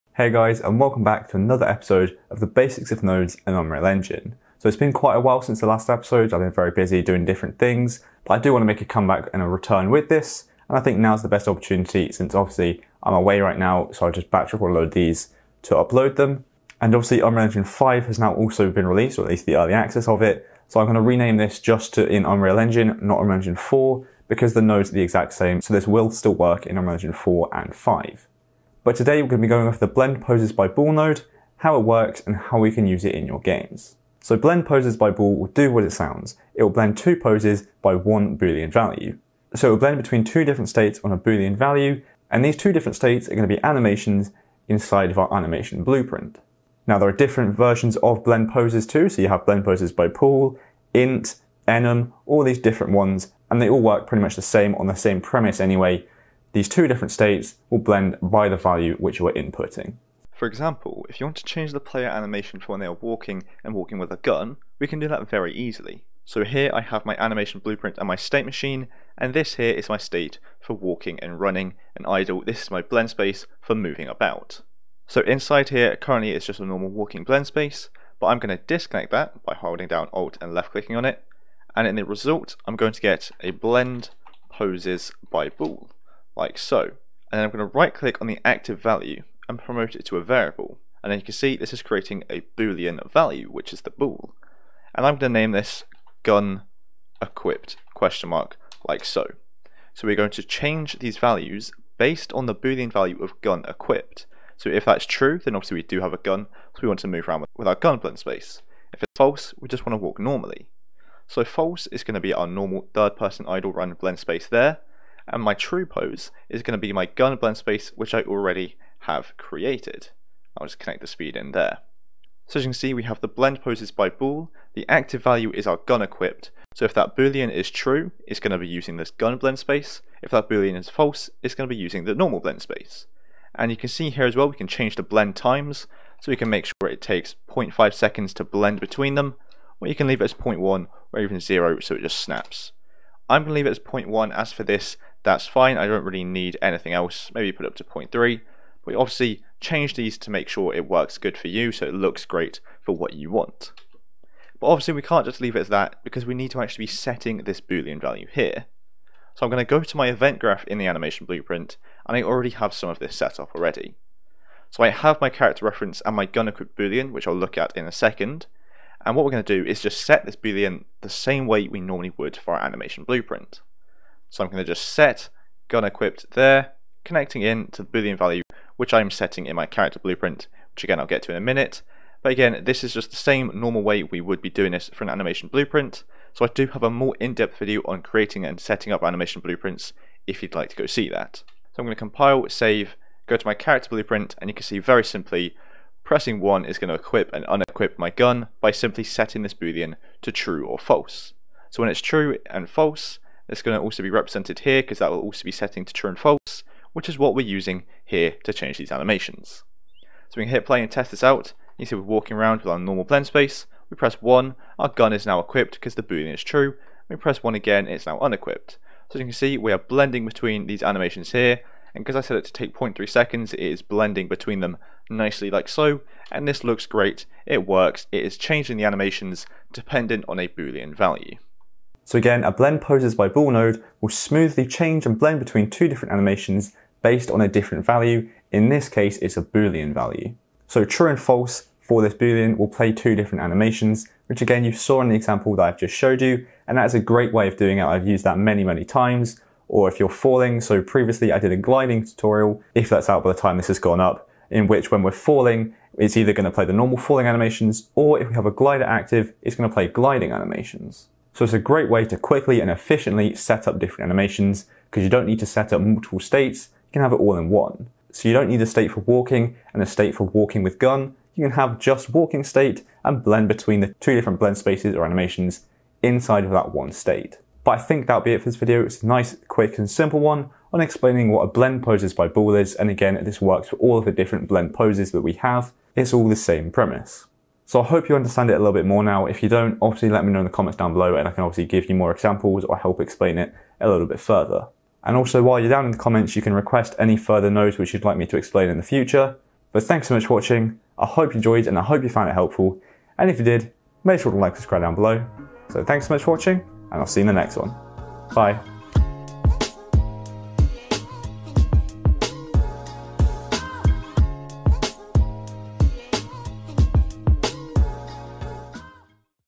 Blend Poses By Bool - The Basics Of Nodes In Unreal Engine
Hey guys, in today's video, I'm going to be showing you what the Blend Poses By Bool node is, how it works and how to use it.